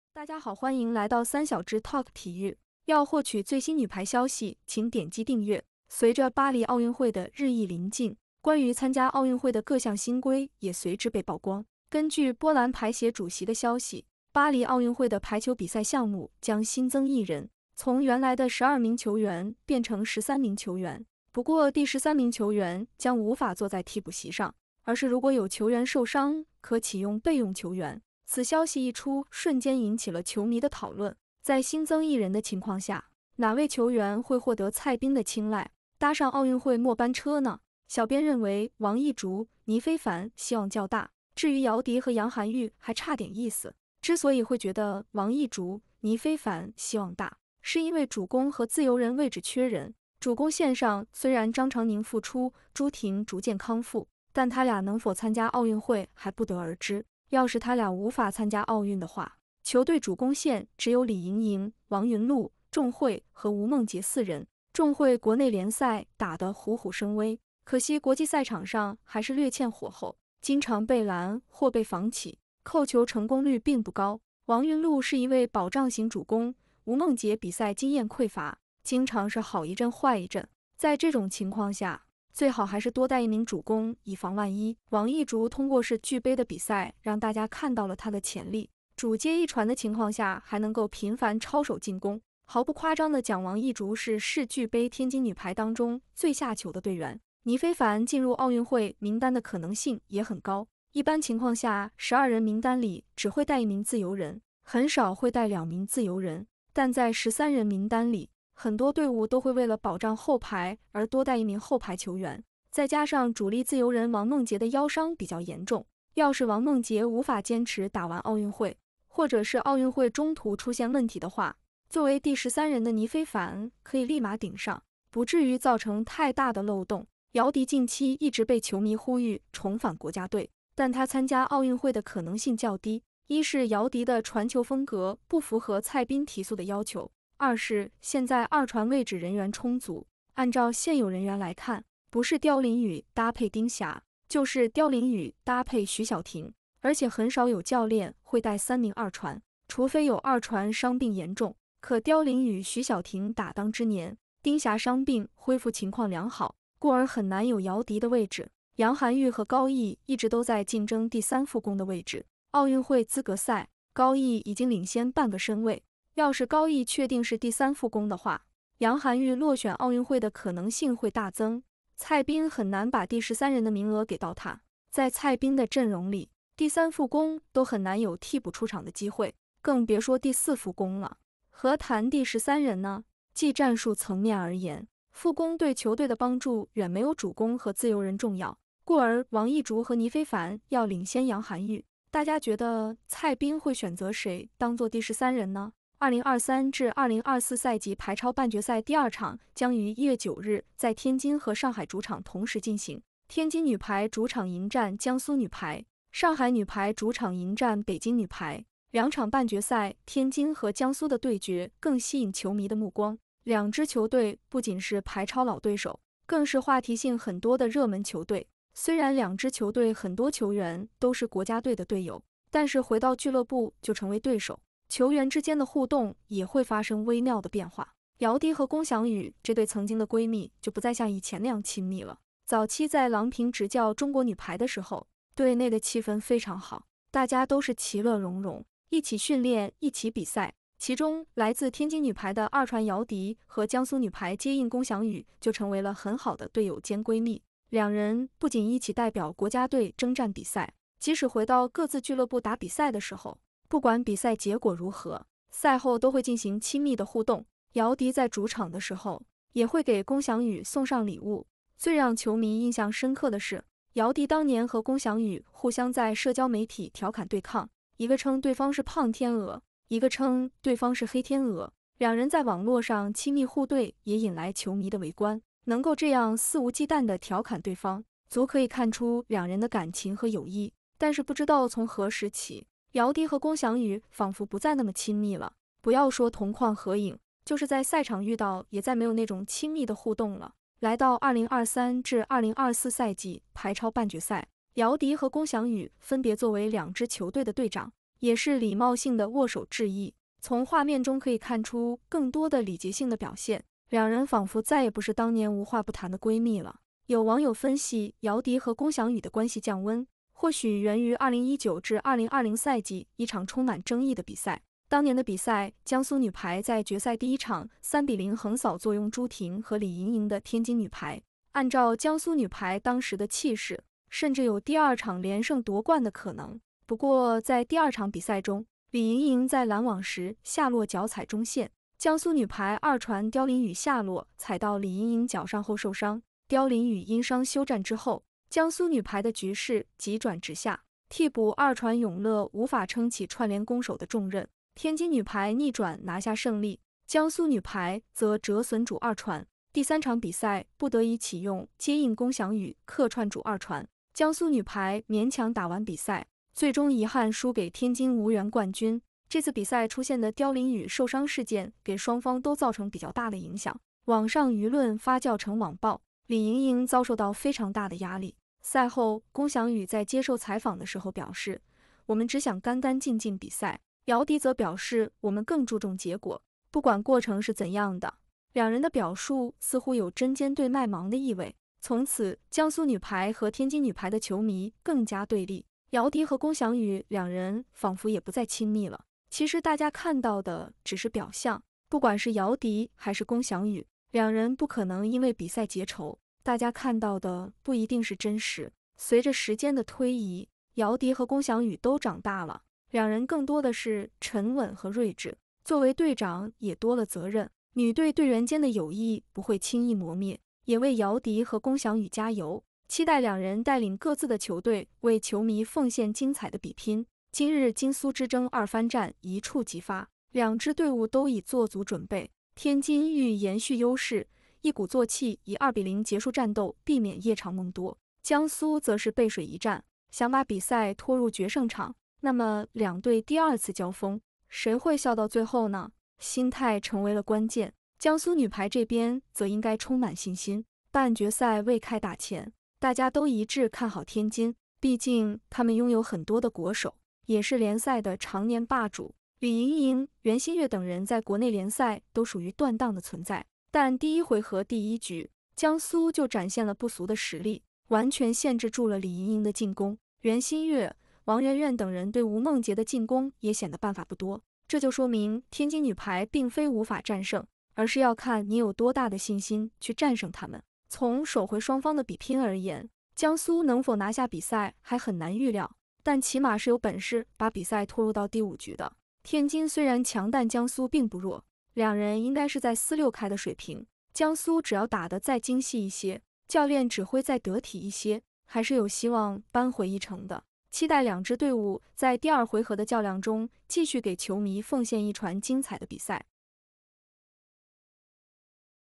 王藝竹迎奧運機會！倪非凡有戲，姚迪差點意思，女排名額增至13人。姚迪龔翔宇，從親密無間到逐漸生疏，小宇當年一句話或是主要原因
在新增1人的情況下，哪位球員會獲得蔡斌的青睞，搭上奧運會末班車呢？筆者覺得王藝竹、倪非凡希望較大，至於姚迪和楊涵玉還差點意思。之所以會覺得王藝竹、倪非凡希望大，是因為主攻和自由人位置缺人。